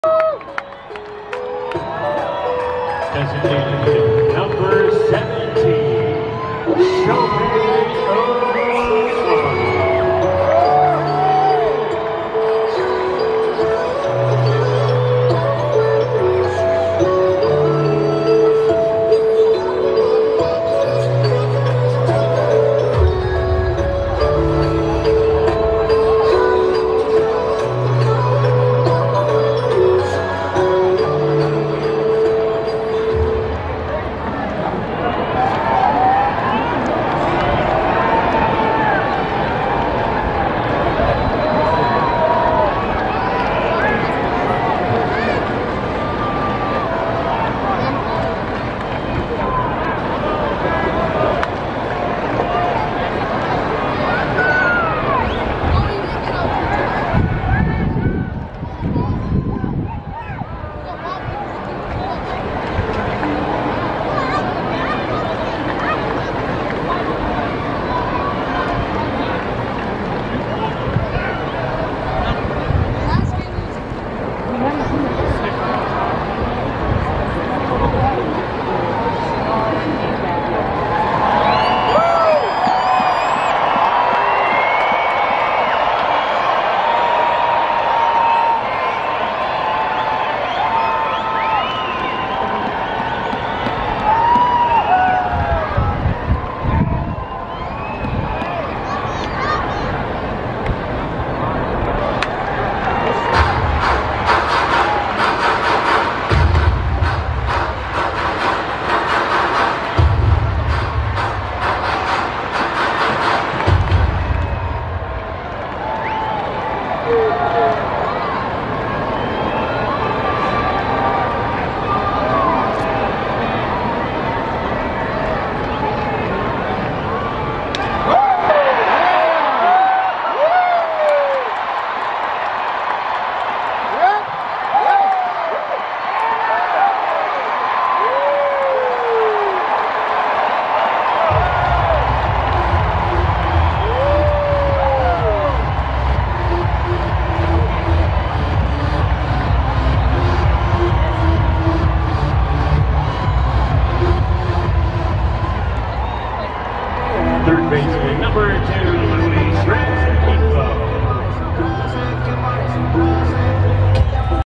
【適時左前打】1点リードの7回1死ランナー1、2塁の追加点チャンスで大谷翔平第4打席！現地映像8月14日
速報大谷翔平ツインズ戦